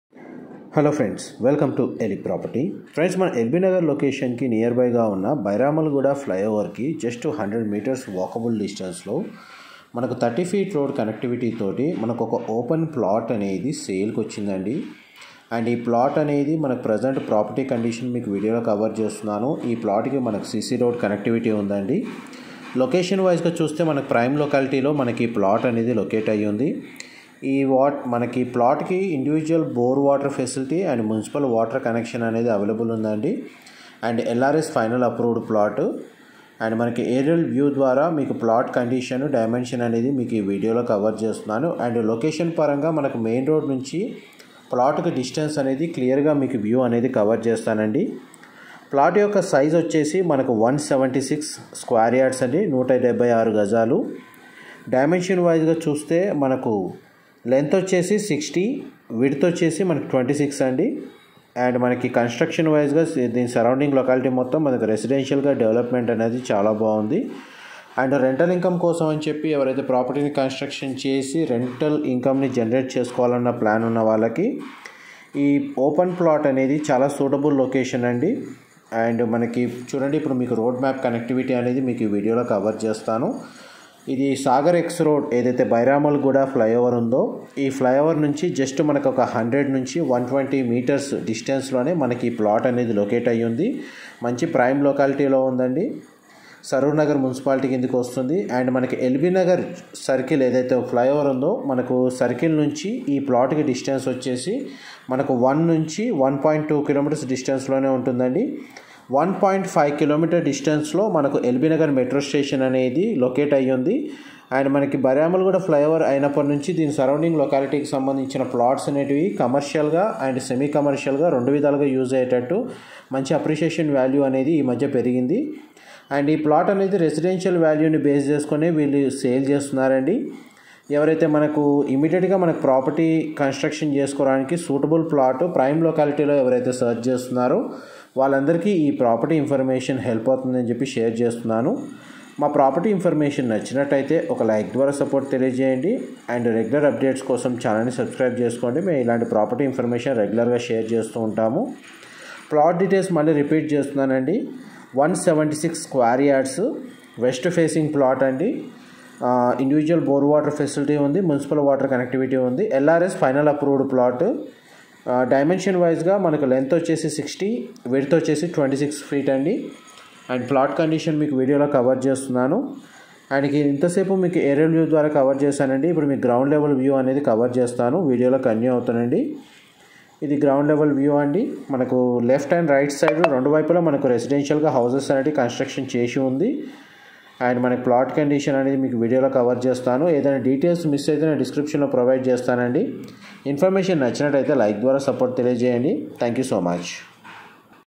L.B.NAGAR MAIN ROAD 176 SQ YARD OPEN PLOT FOR SALE HYDERABAD ELIP PROPERTY
OPEN PLOT FOR SALE NEAR SAGAR X ROADS MAIN ROAD LB NAGAR HYDERABAD ELIP PROPERTY

PLOT AREA : 176 SQ YRD
FACING : WEST FACING
DIMENTION : 60 * 26 FT
LRS FINAL APPROVED PLOT
PRICE : 98 K INR PER SQ YRD